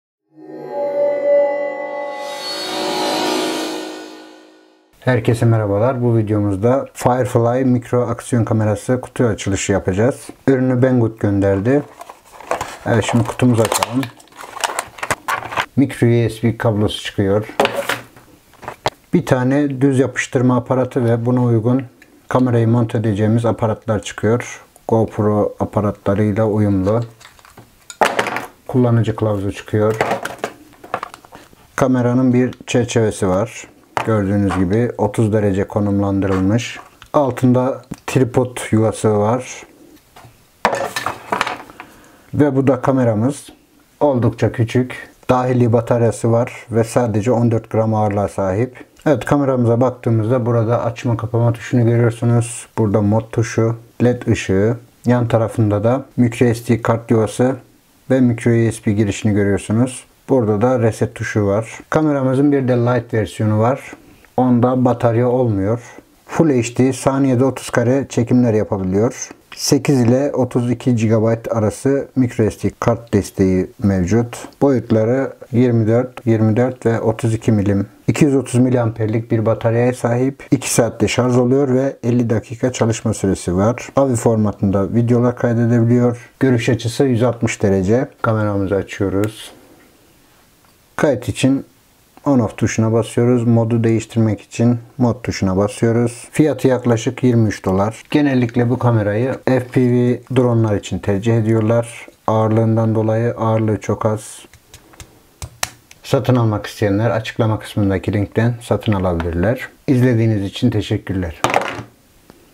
Hawkeye FIREFLY Mikro Aksiyon Kamerası
Hawkeye FIREFLY Mikro Aksiyon Kamerası Kutu Açılışı
Kupon kodu:BGTechnoDeli3

Özellikler
Çözünürlük: 1080P 30FPS/720P 30FPS
Micro SD kart 8-32G
Ebat: 24×24×32 mm
Ağırlık: 14g
Batarya: 200mAH
Çalışma süresi: 50 dakika
Şarj süresi: 2 saat
Dosya formatı: AVI
Lens: F/2.8
Görüş açısı: 160 derece